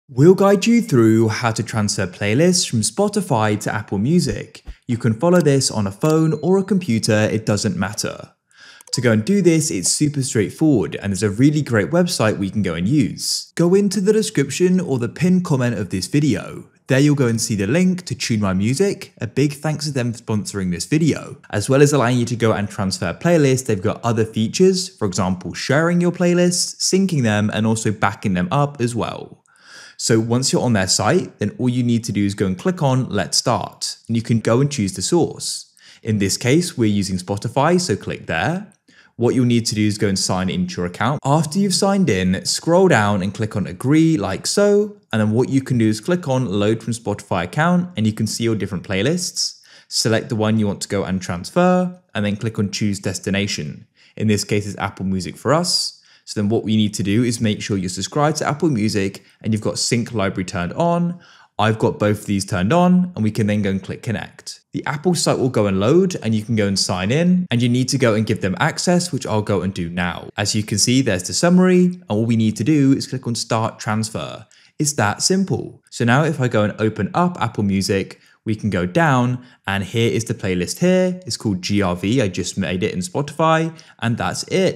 How To Transfer Spotify Playlist To Apple Music - iOS, Android, Mac and Windows
Learn how to transfer spotify playlist to apple music in this video.

GuideRealm